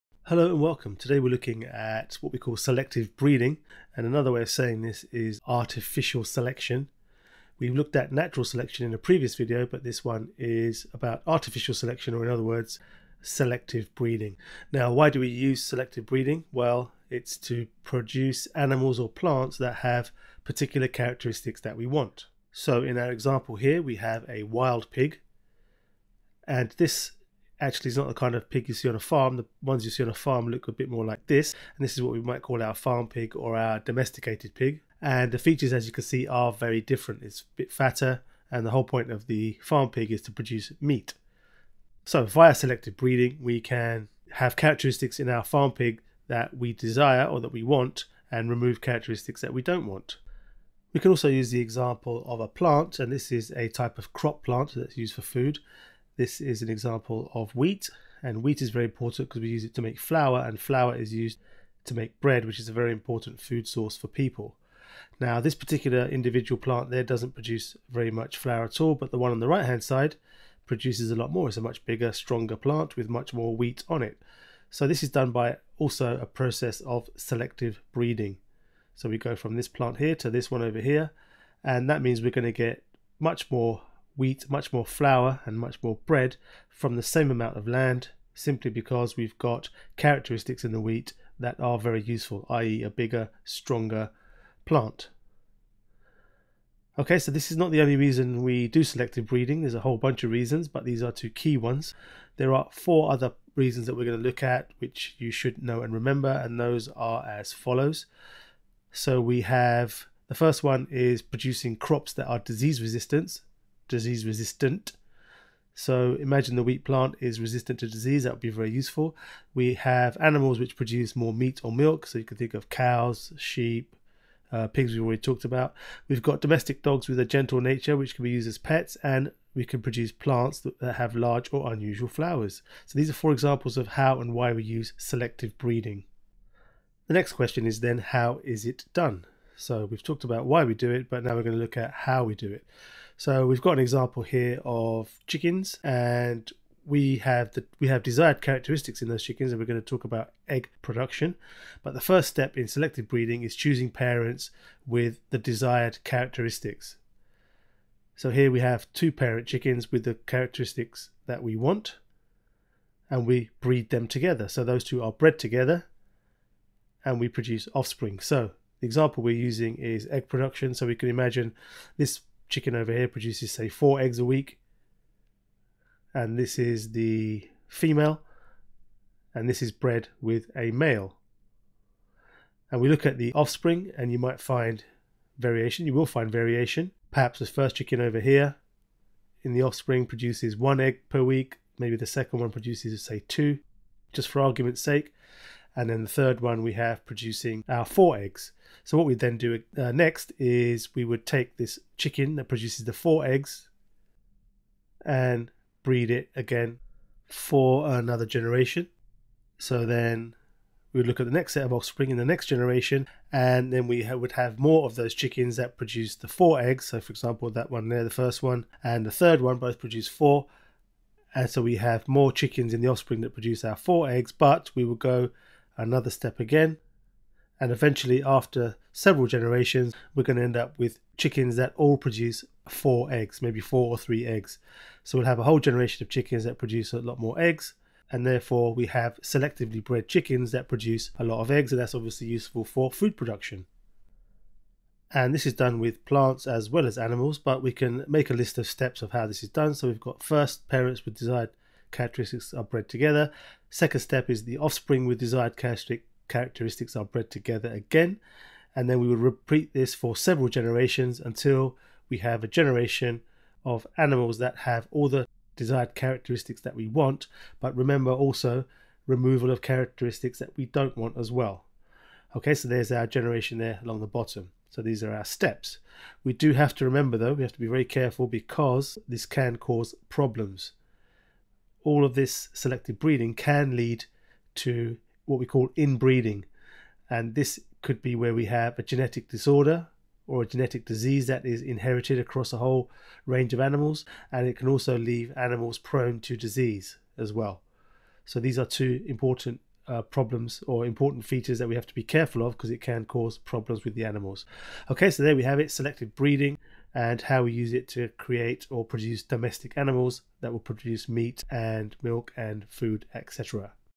Edexcel GCSE Science and Biology - Selective Breeding
We explain selective breeding and its impact on food plants and domesticated animals.

The videos are planned, recorded and edited by science teacher, Kishore Vyas.